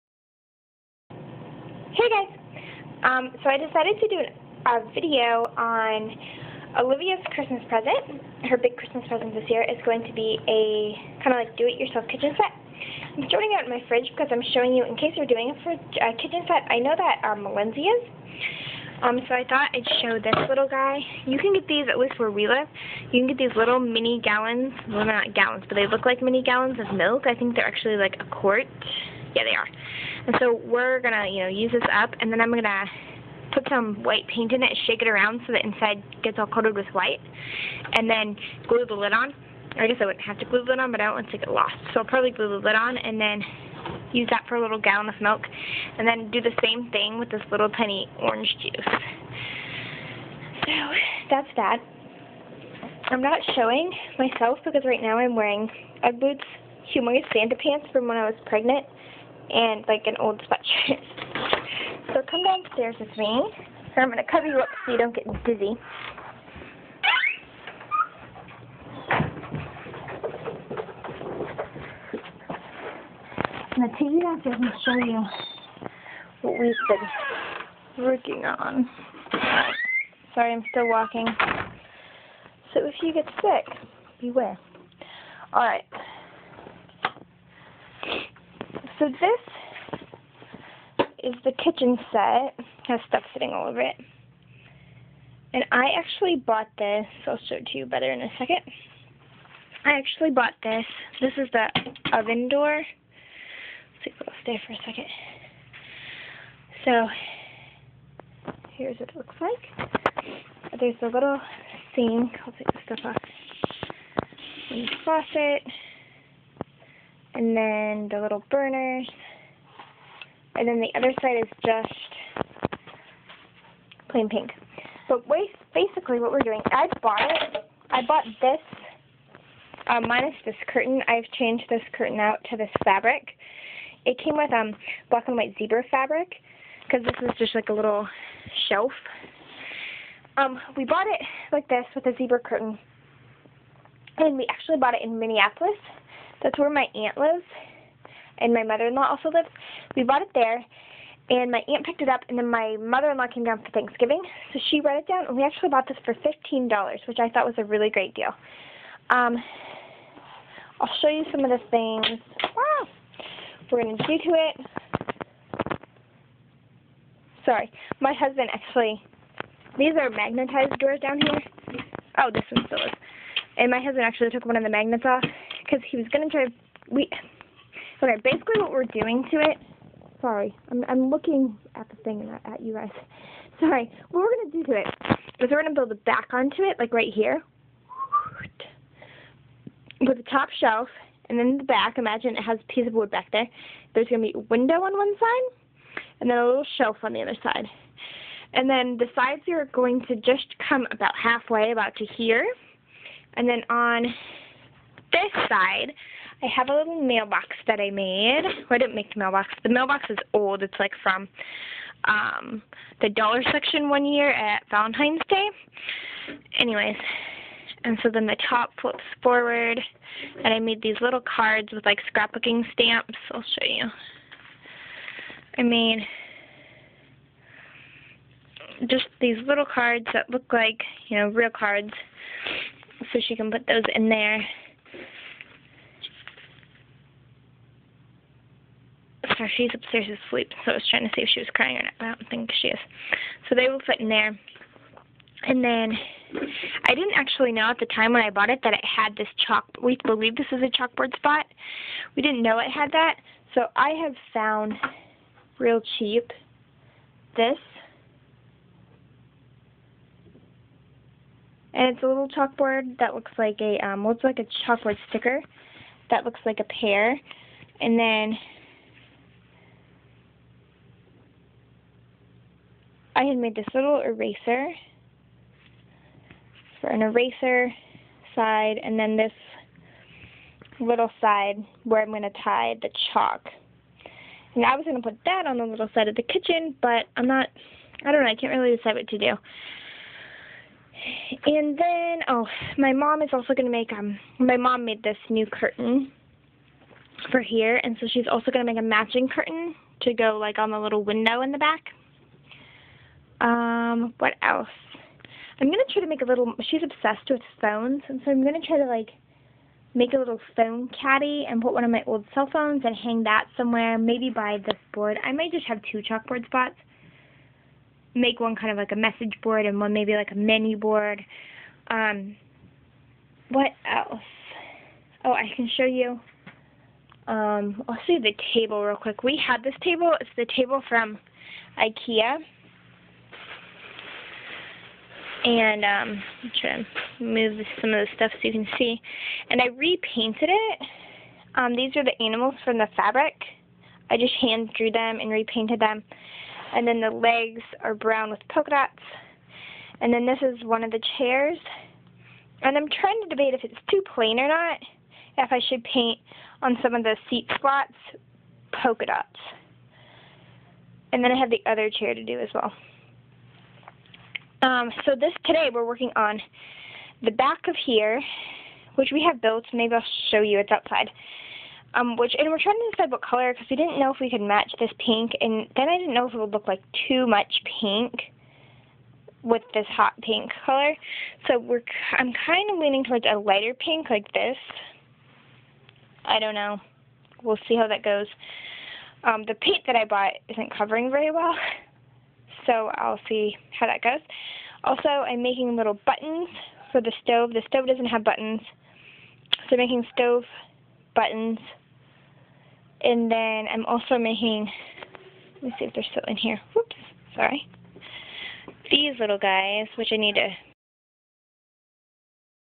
DIY play kitchen pt 1
This was filmed in the earlier part of Dec. It is the before video! Coming soon the after of the kitchen!